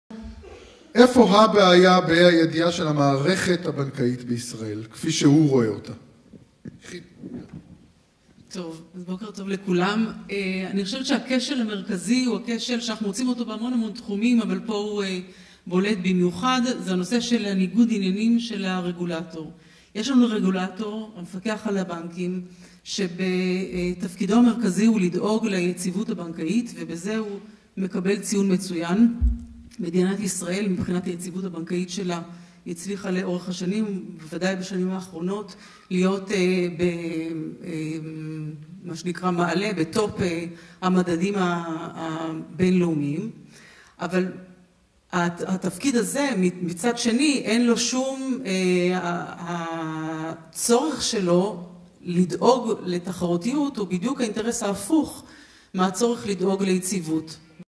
ניגוד העניינים המובנה של הרגולטור בפיקוח על הבנקים
עו"ד נילי אבן חן, סמנכ"לית כלכלה ומחקר בתנועה על ניגוד העניינים המובנה של הרגולטורים בפיקוח על הבנקים בישראל.  הדברים במסגרת פאנל מיוחד ביום איכות השלטון - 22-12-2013